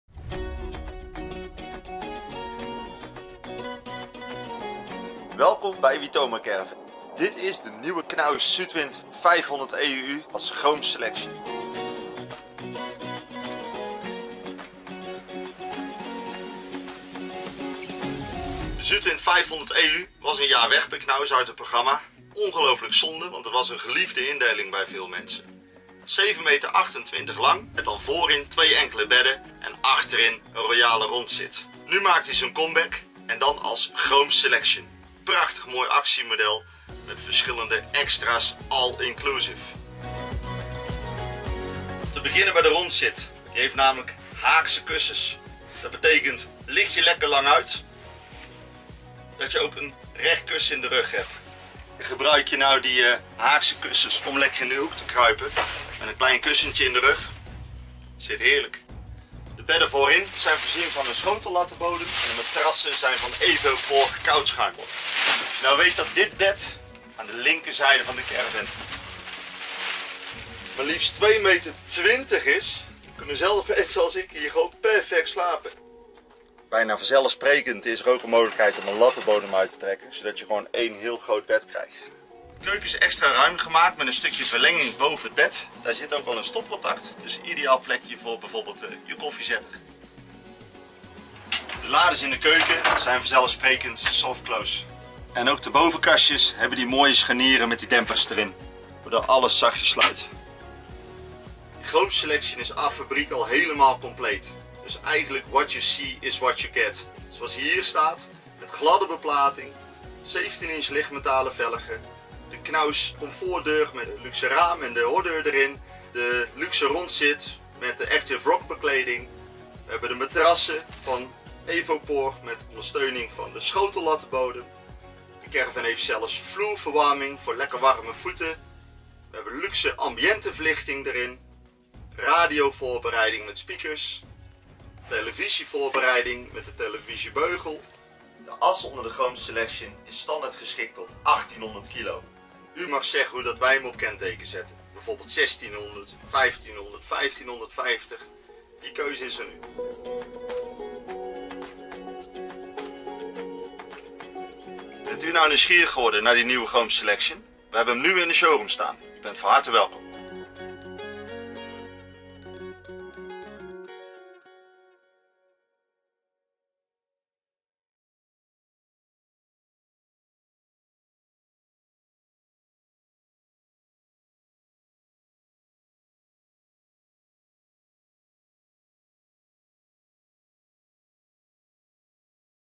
Knaus Sudwind 500EU Chrome Selection model 2020 bij Witoma Caravan
De vertrouwde Knaus Sudwind 500 EU is terug in een nieuw jasje, de Chrome Selection, nu te zien bij Witoma Caravan!

Witoma Caravan BV
Friezenstraat 2-b
5249 JS Rosmalen